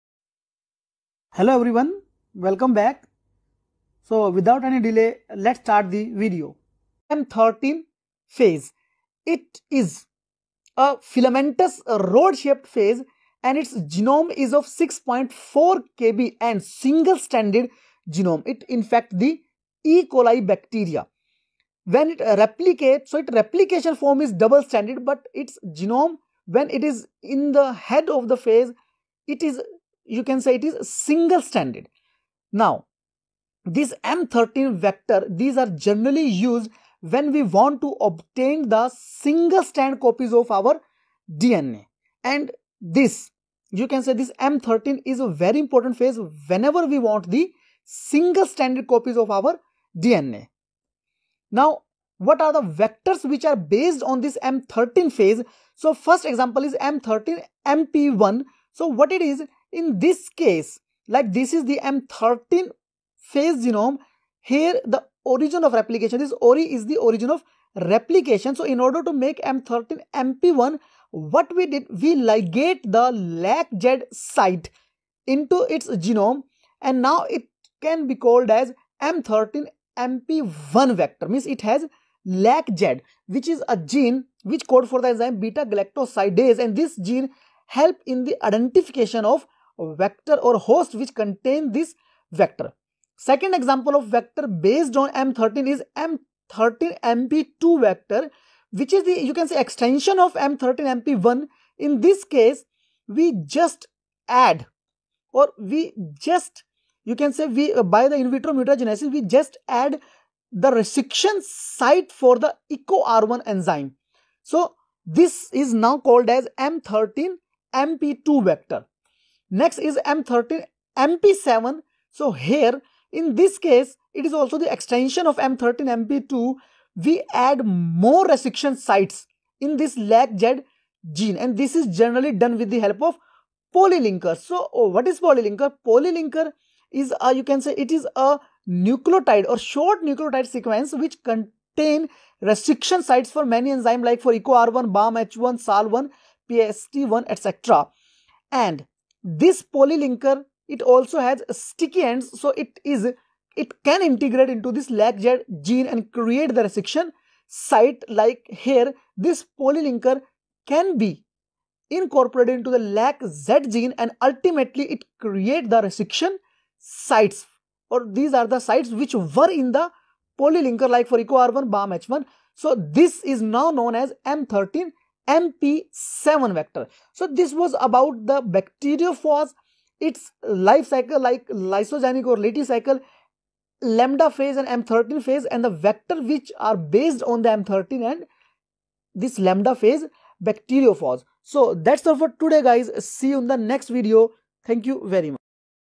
M13 phage cloning vectors | M13mp1 | M13mp2 | M13mp7 | Biotechnology
In this video you will learn about M13 phage cloning vectors

M13mp1 | M13mp2 | M13mp7